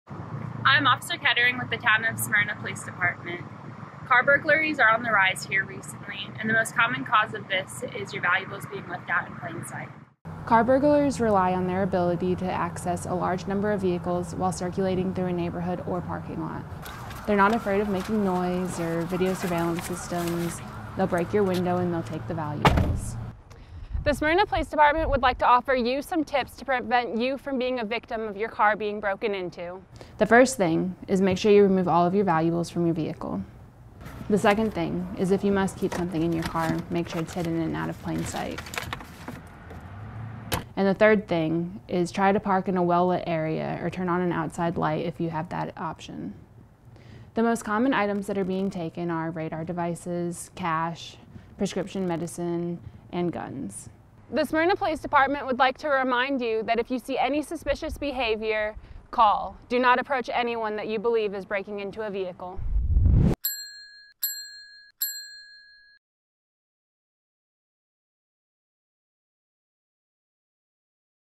PSA - Car Break-ins - Town of Smyrna Police Department
The Town of Smyrna Police Department offers several effective steps you can take to help deter and prevent car break-ins.